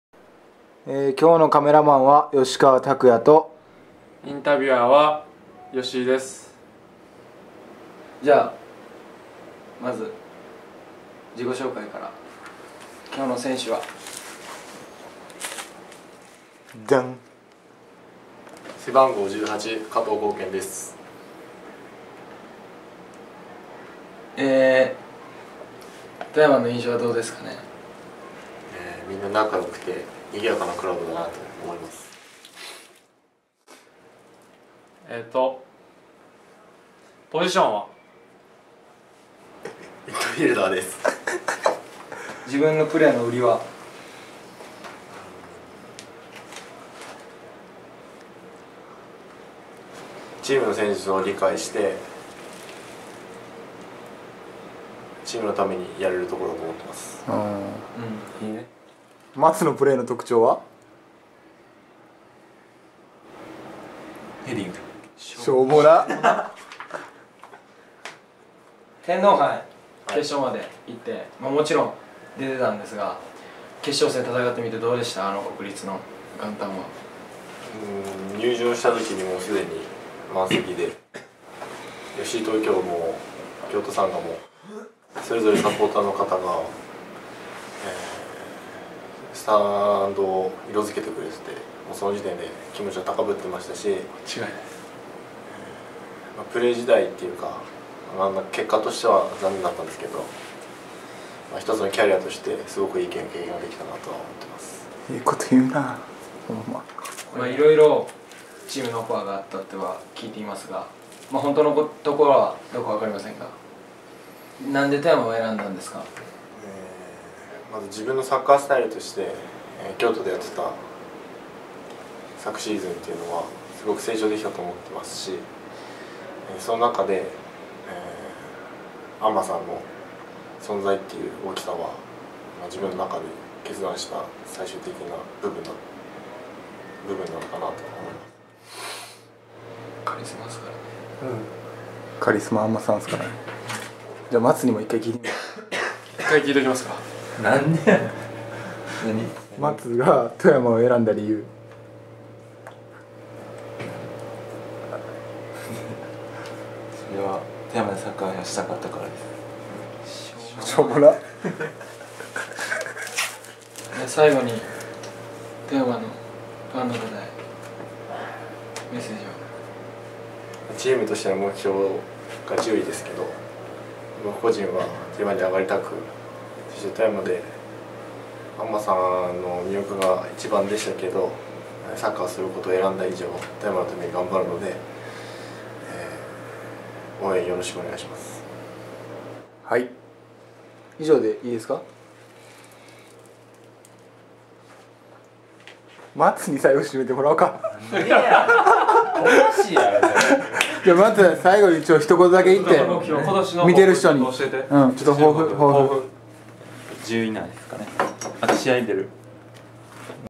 0125静岡キャンプ8日目.wmv
2012/01/25静岡キャンプ8日目は新加入選手の最後の紹介です。